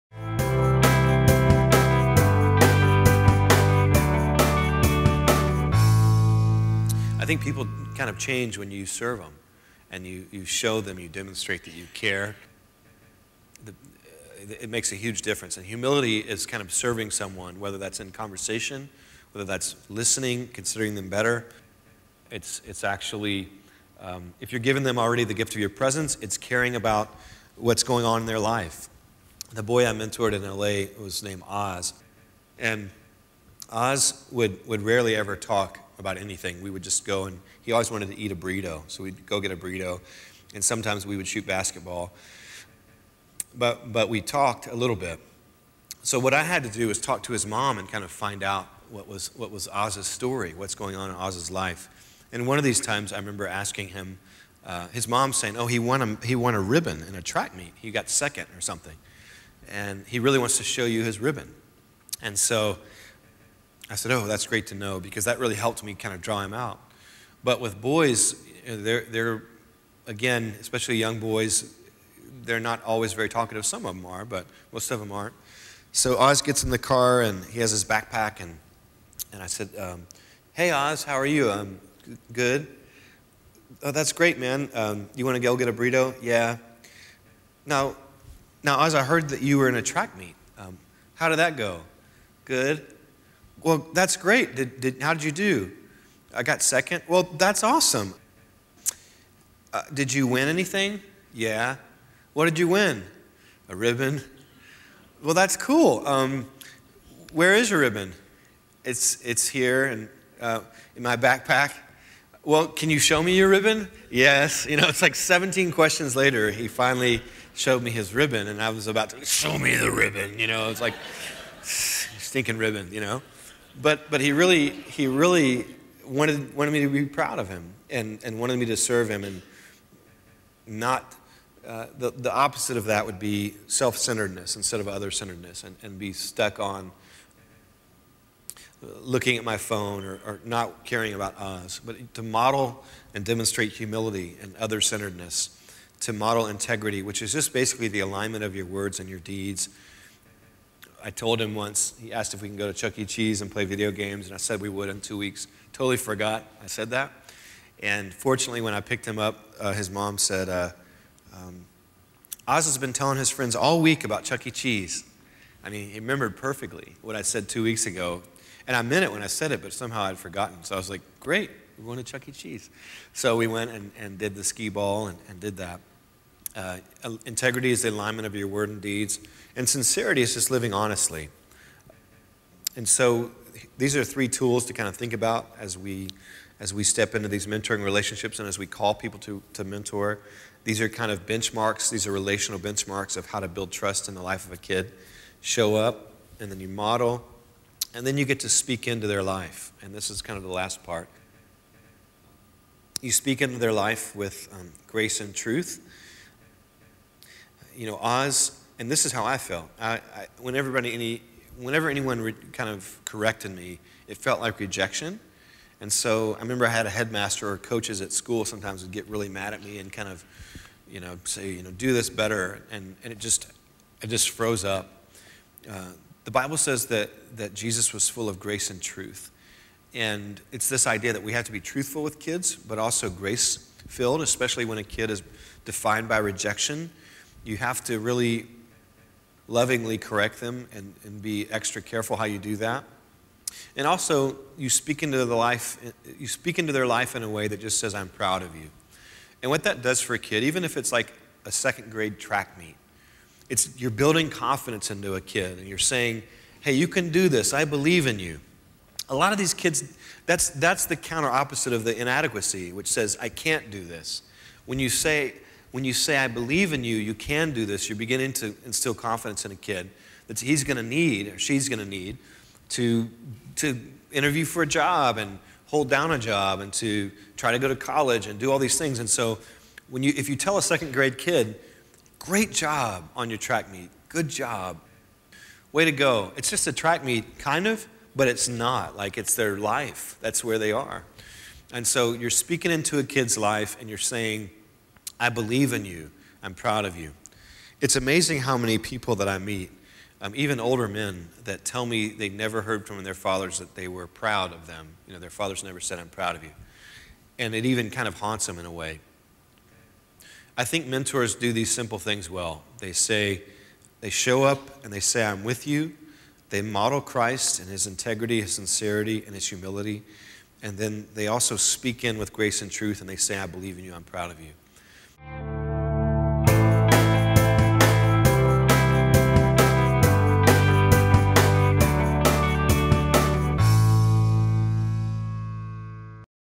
the Y & The Mentoring Project Event - Dr. John Sowers on Oz's Story
Hear Dr. John Sowers talk about the basic foundations of mentoring.  Excerpt from "Curing the Epidemic" event hosted by the Carlisle Family YMCA at the Carlisle Brethren in Christ Church in April, 2013.